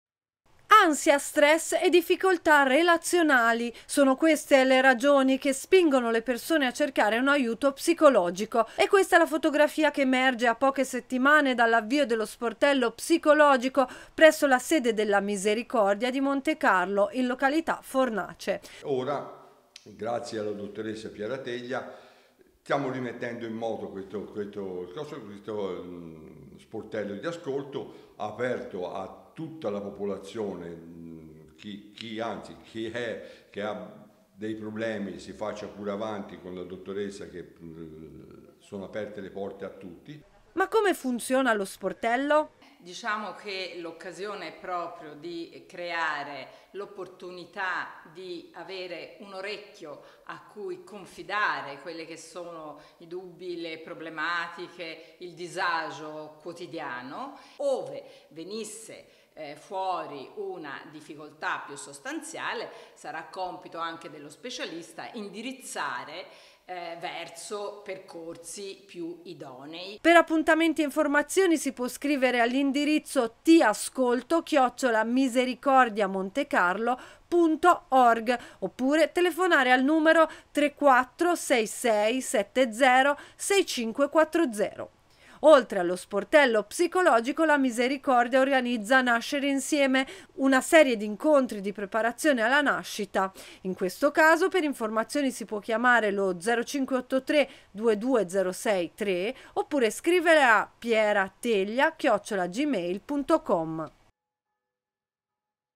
Alla Misericordia di Montecarlo gli "sportelli psicologici"
La Misericordia di Montecarlo con il suo staff di psicologhe offre gratuitamente alla cittadinanza un punto di ascolto psicologico e uno sportello dedicato alla prenatalità.
NoiTv